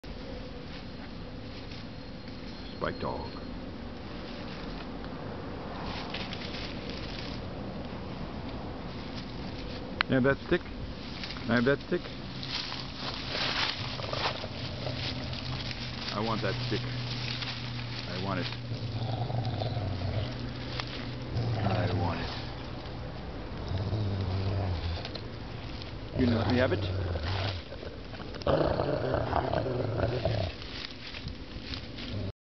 Labrador / Rottweiler mix rolling in Laguna Beach Ivy
This is my brother-in-law’s 120 pound dog “Spike” doing what he likes best!